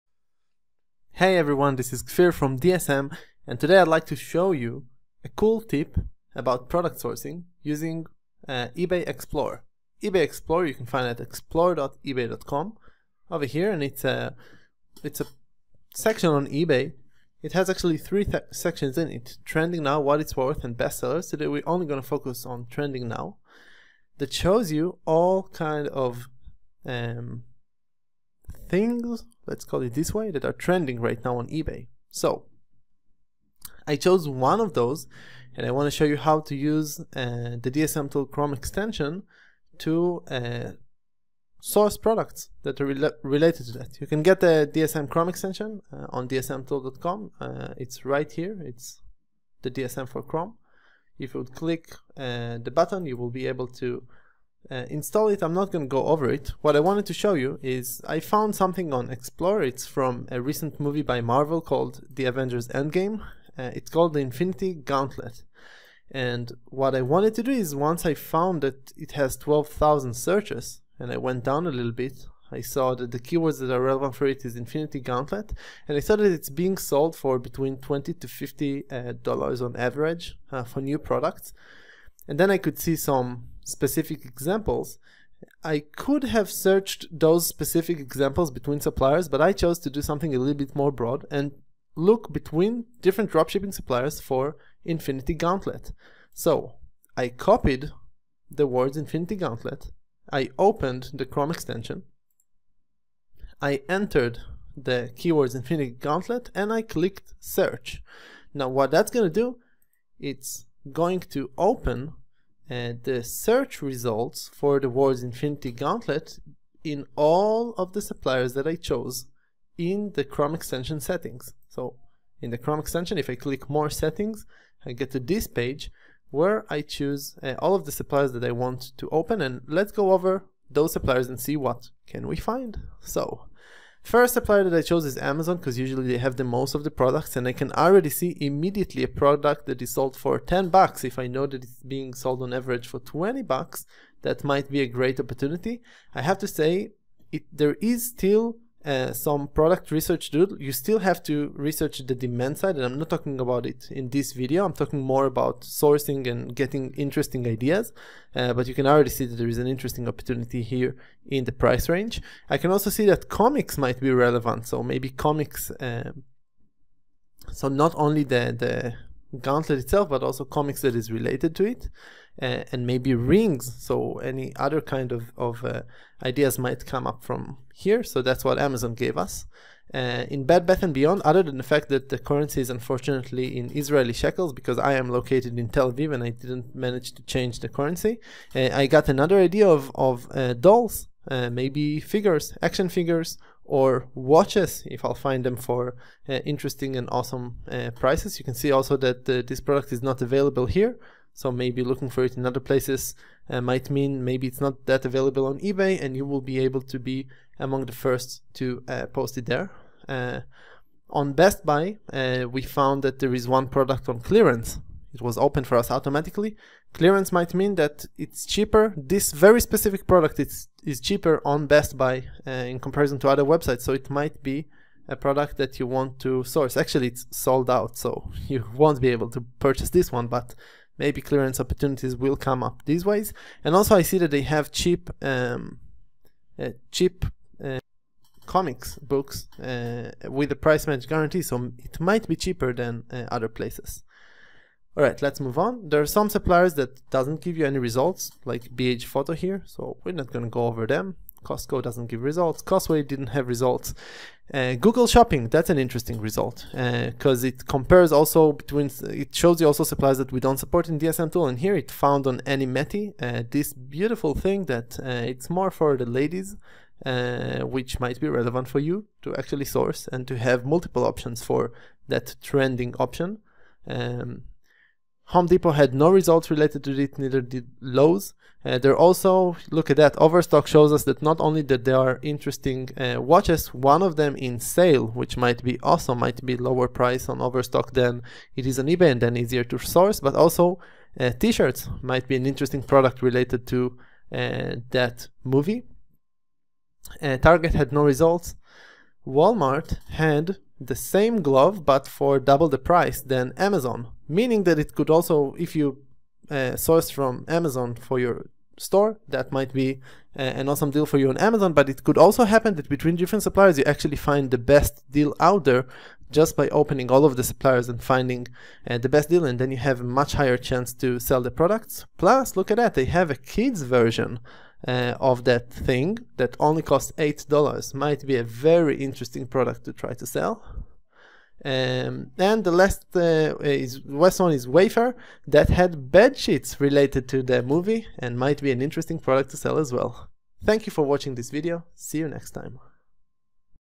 Drop shipping product sourcing with eBay explore
Find products for drop shipping is a challenging task. In this video, we will use eBay explore feature to find the demand for products and the DSM Product Search feature to look for potential suppliers for those products for your dropshipping store.